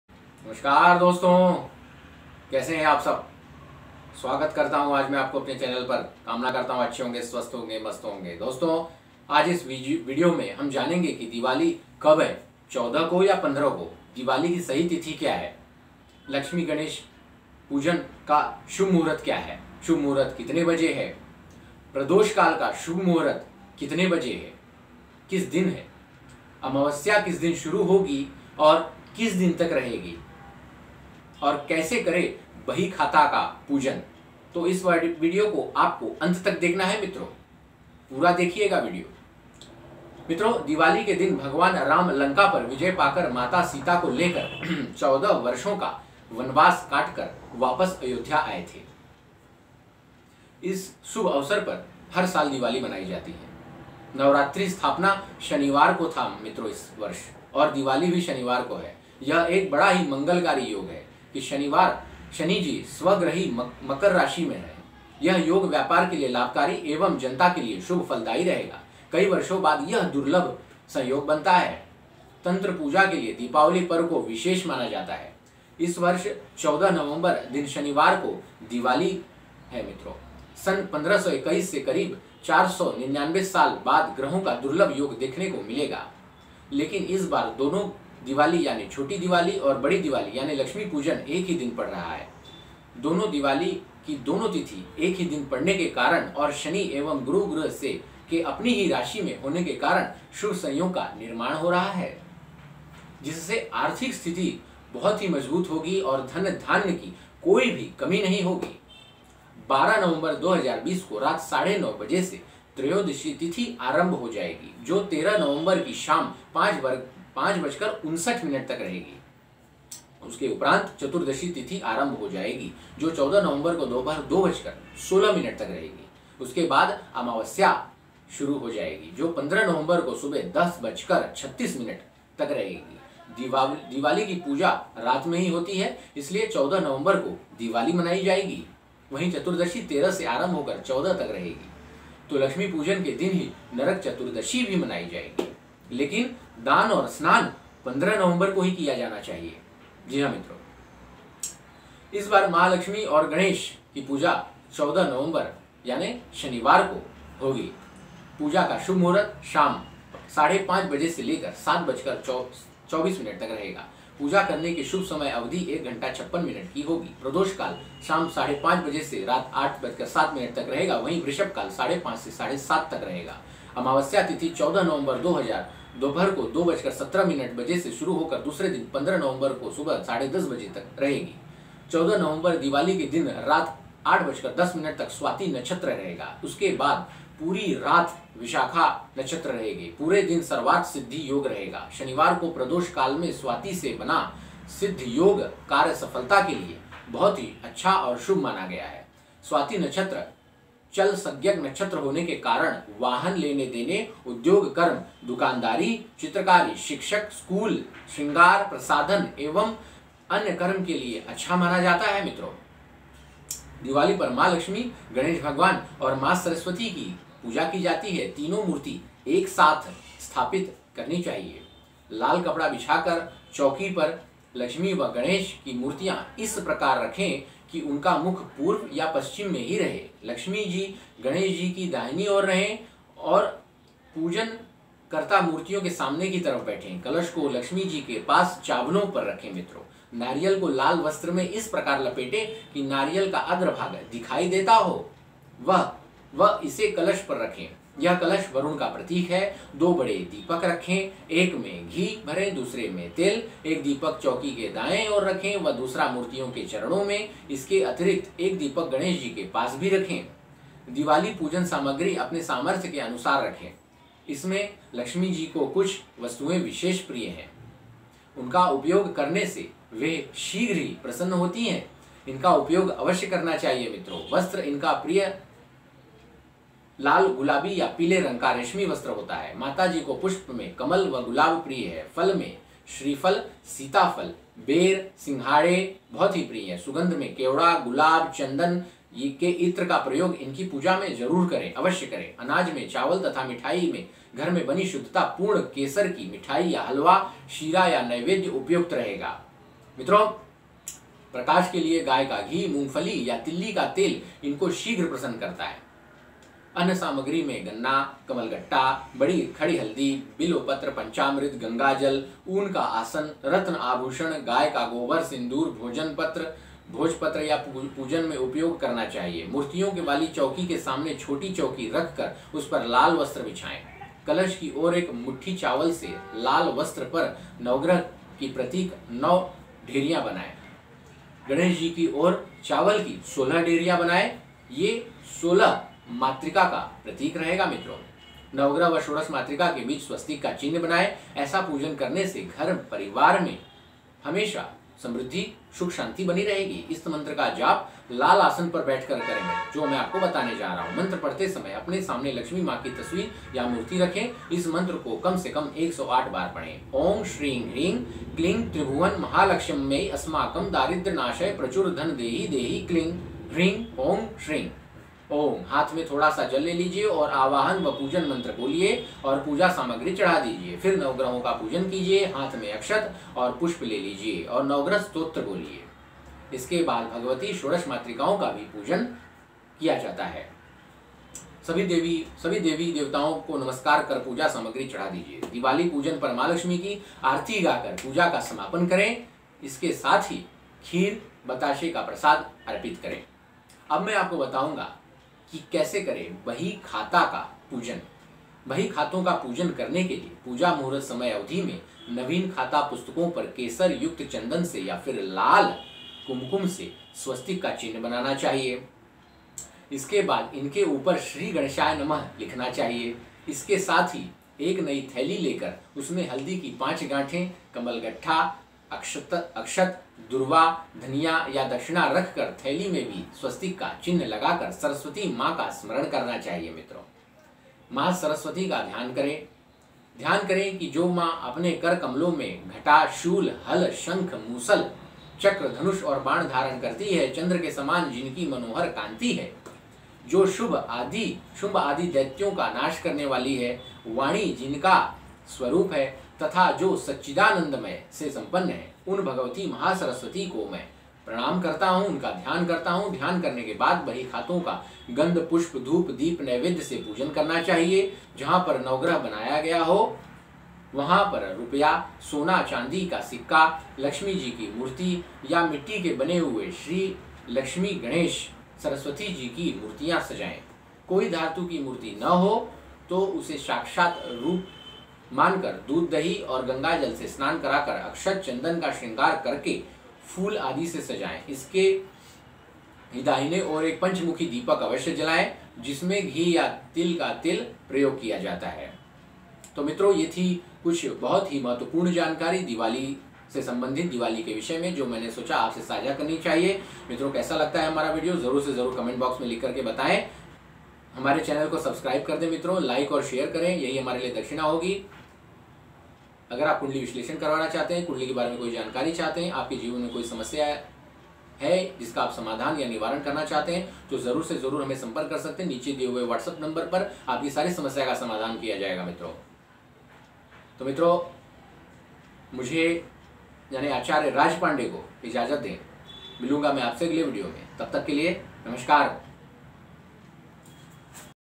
Diwali kab hai 2020 | Lakshmi puja kab hai | Lakshmi puja vidhi | Diwali puja samagri
Is video mein hum janenge ki Diwali kab hai 2020, Lakshmi puja kab hai, Lakshmi puja vidhi, Diwali puja samagri, Diwali 2020, Main hun Acharya Raj pandey, Whatsapp